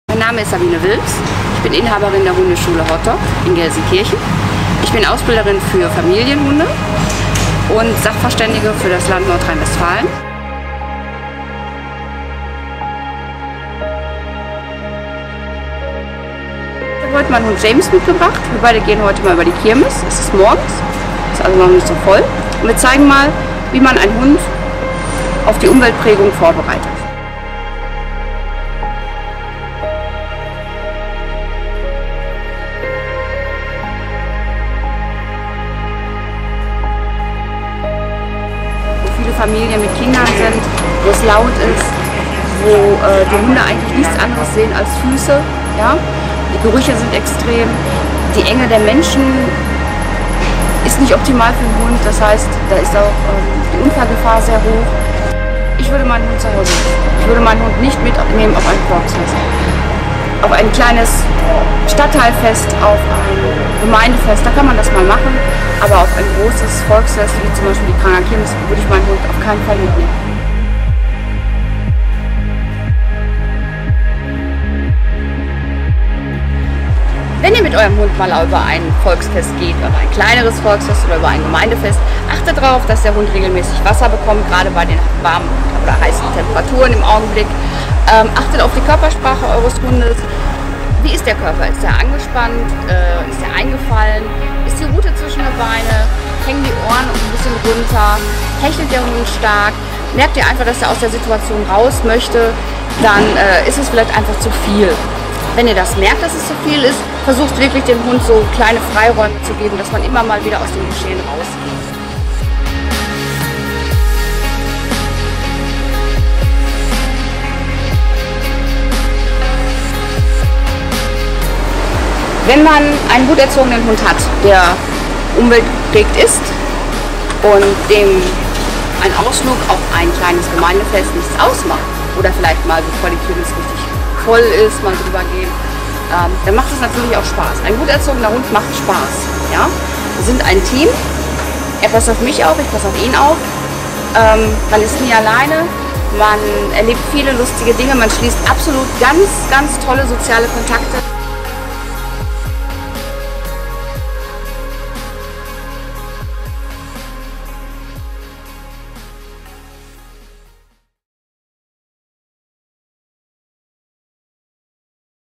Wie man Hunde auf Umwelteinflüsse vorbereiten kann | *Thema: Volksfeste*
Zwischen der aufregenden kirmeszeit auf der Cranger Kirmes und dem weihnachtsmarkt in der innenstadt von herne gibt es viele feste zu feiern! Eines davon ist der feuerabend am kommenden Samstag, 21. September 2019! 💥😍 Auch hunde werden mitunter zu solchen Gelegenheiten von ihren Besitzern mitgenommen. Doch damit ein gemeinsamer Besuch auf einem fest nicht zu einer größeren Belastung für das tier wird, gibt es einiges zu beachten: Wir haben Sabine Wilms von der Hundeschule Hot-Dog für einen Tag auf die Cranger Kirmes begleitet. Mit Hund James und Kirmeshund Luna zeigt sie, worauf man achtgeben sollte. Alle infos zum Feuerabend findet Ihr natürlich auf unserer